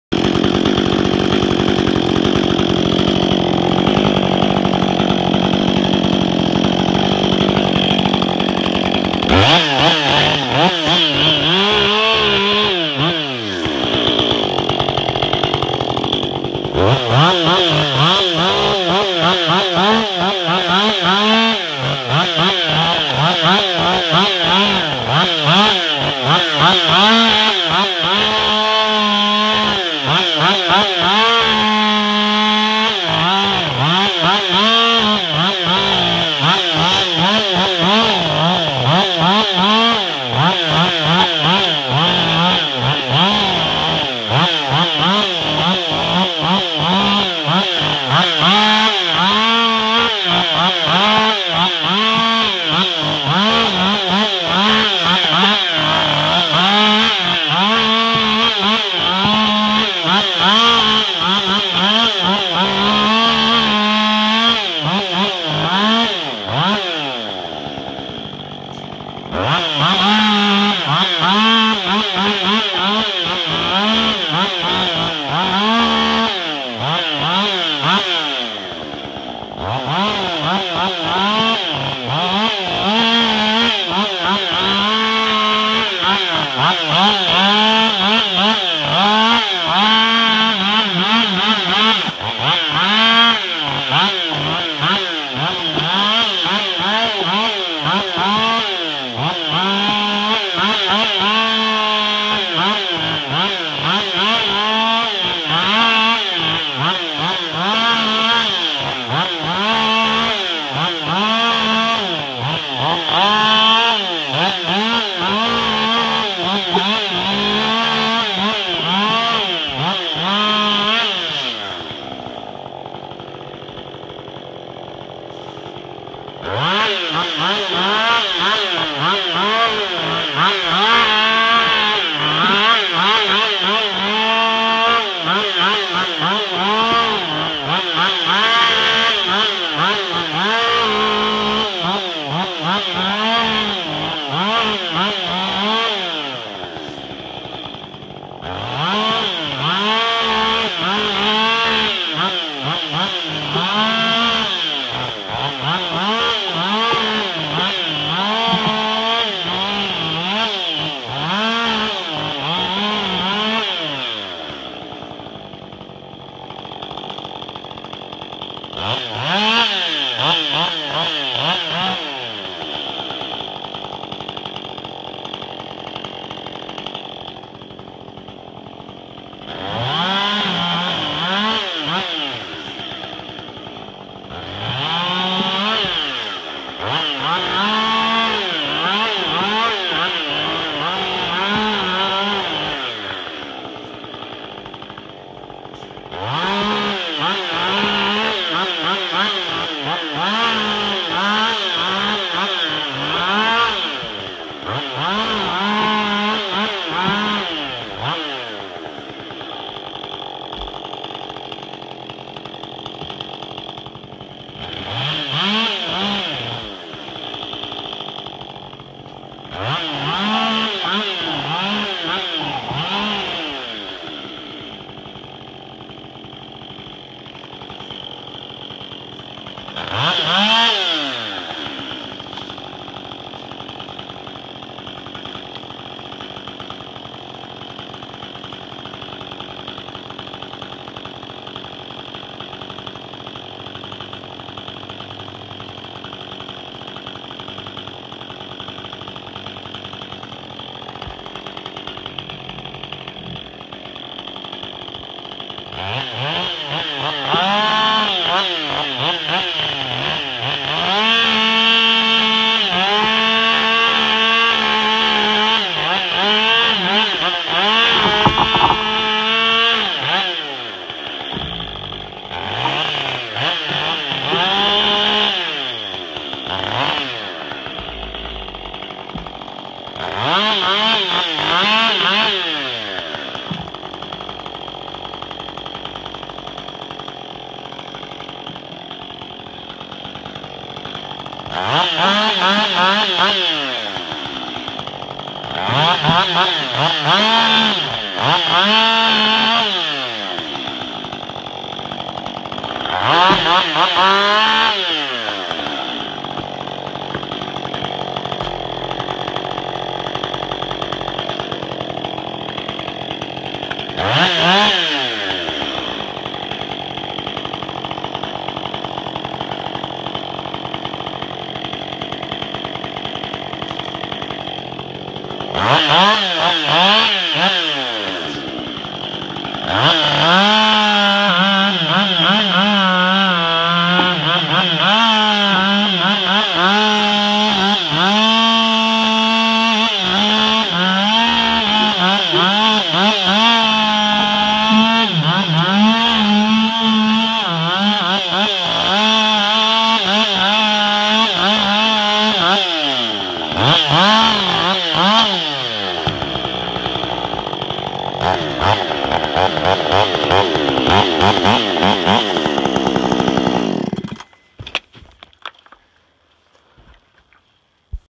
Limbing a Western Red Cedar
Limbing up a Cedar I dropped. Husky 562XPGW ported with heated wrap bars and a 28" Oregon reduced weight bar.